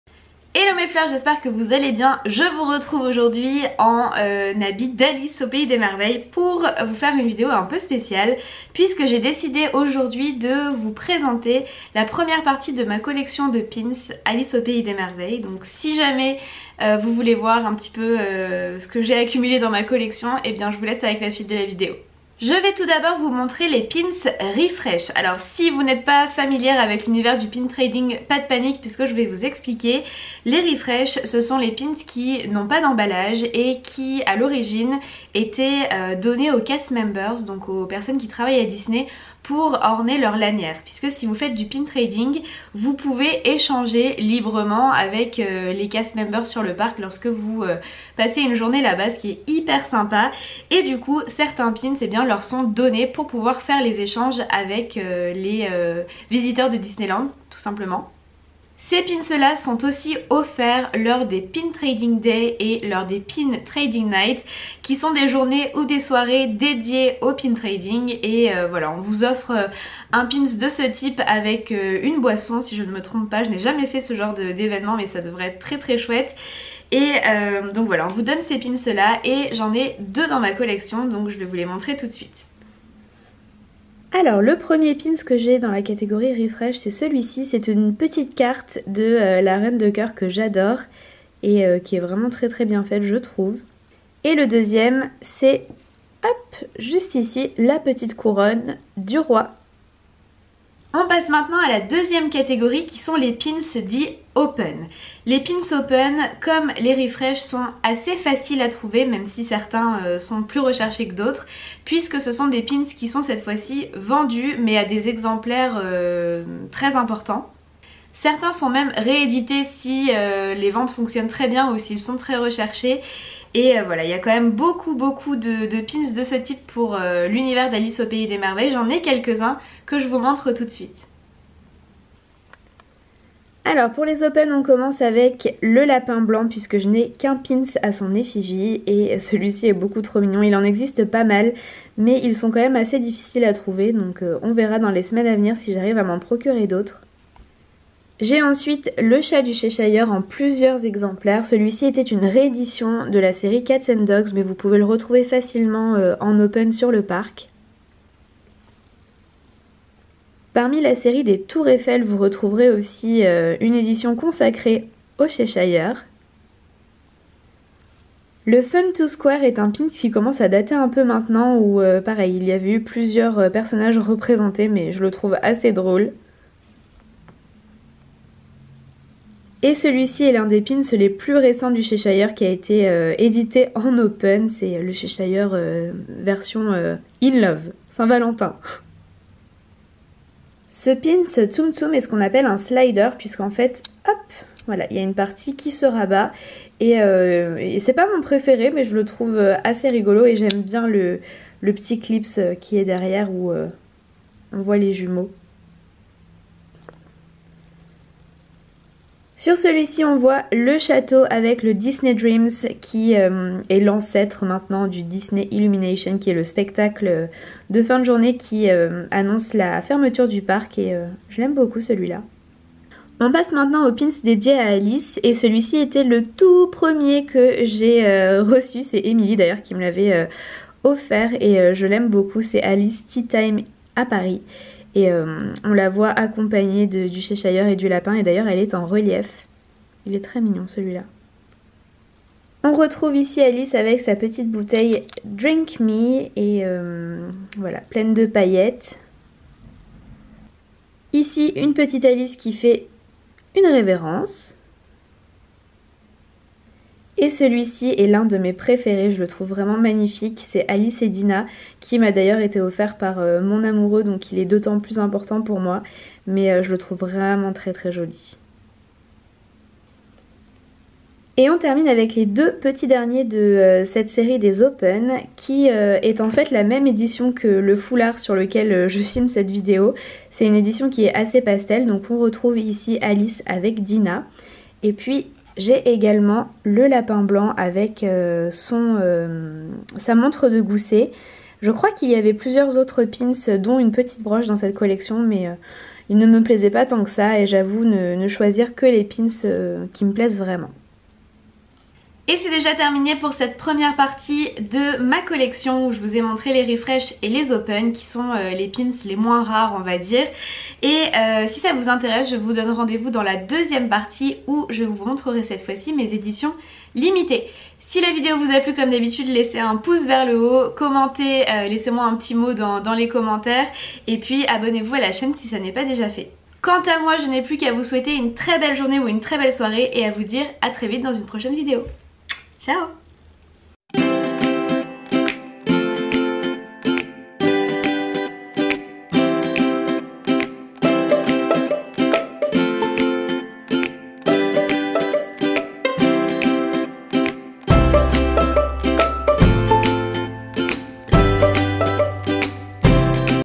MA COLLECTION DE PINS DISNEY #1
♡ Hello mes fleurs ! Le carnaval arrive bientôt ! À cette occasion, j’ai renfilé mon costume d’Alice et j’ai décidé de vous présenter la première partie de ma collection de pins Disney ! J’espère que cela vous plaira ! Bon visionnage !

♡ Rejoignez la communauté des fleurs en vous abonnant à la chaîne C’est gratuit et ça me donne le sourire !

Montage : Gaëtan Boyron.

Soyez spectatrices,
Fleur.